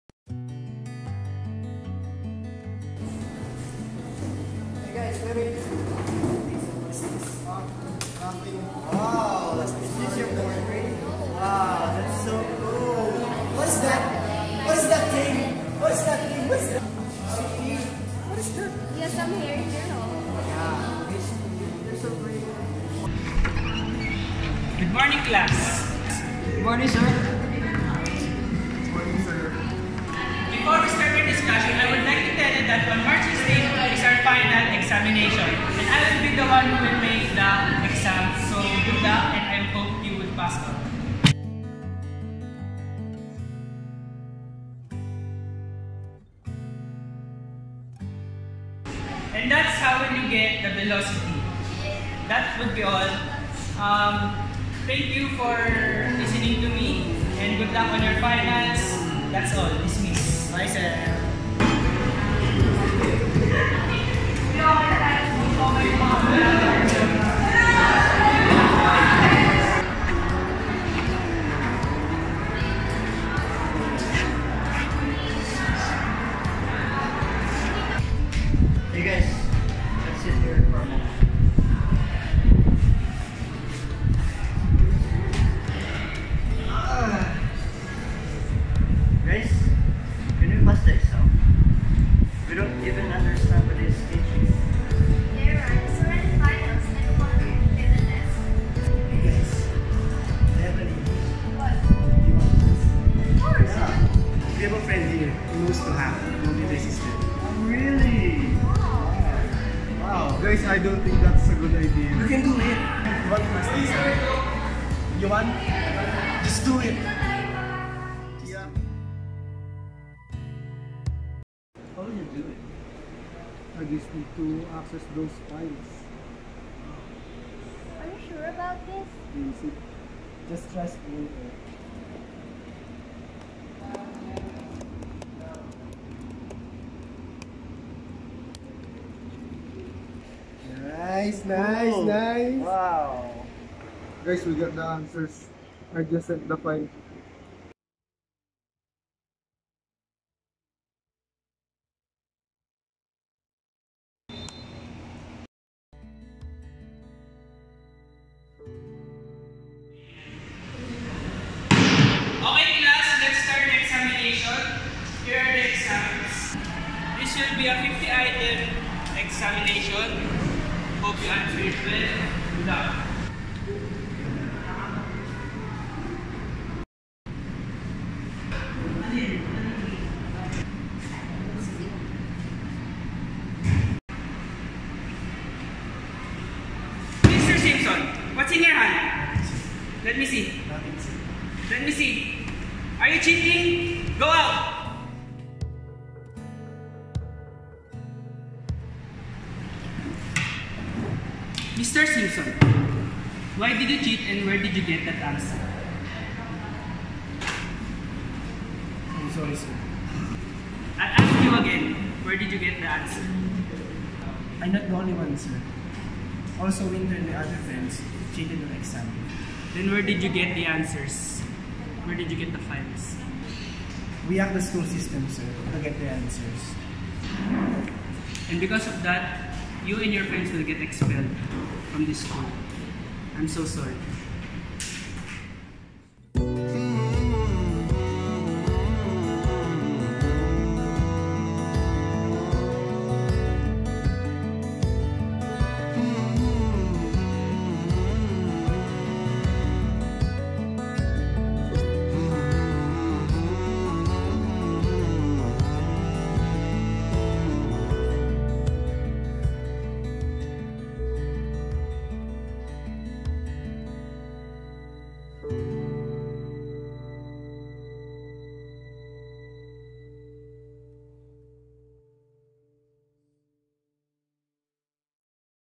"ANSWERS" HACKING IS A CRIME
A SHORTFILM ABOUT 4 STUDENTS WHO CHEATED ON THE EXAM BY HACKING THE SCHOOL SYSTEM AND GETTING THE ANSWER KEYS OF THE EXAM.
PRESENTED BY AR12FA1
GALO ROY
GOTERA MARIA
SUMMERFIELD ANDRE
LORENA HOMER
CADELINA VINCENT